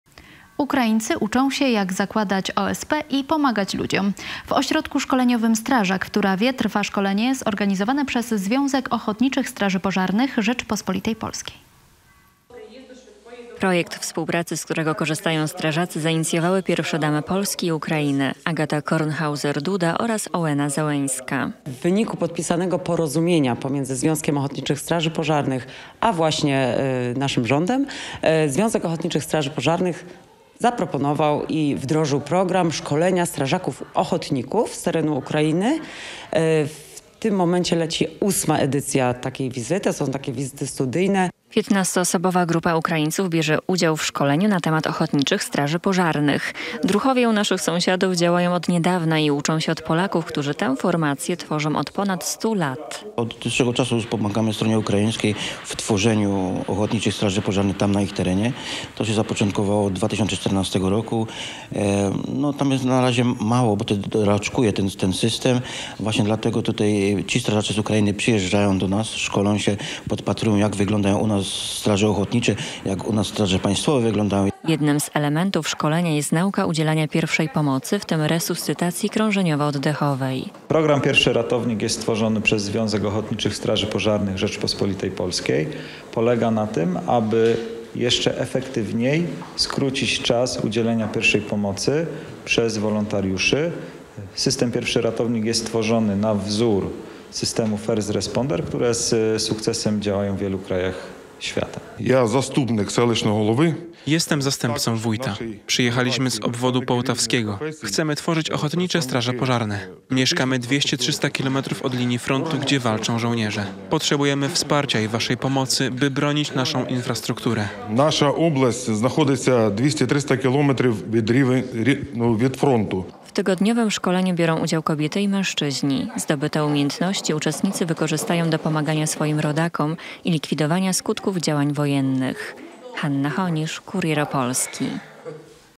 Jak zakładać OSP? Szkolenie dla Ukraińców
Ukraińcy uczą się jak zakładać OSP i pomagać ludziom. W Ośrodku Szkoleniowym "Strażak" w Turawie trwa szkolenie zorganizowane przez Związek Ochotniczych Straży Pożarnych Rzeczypospolitej Polskiej.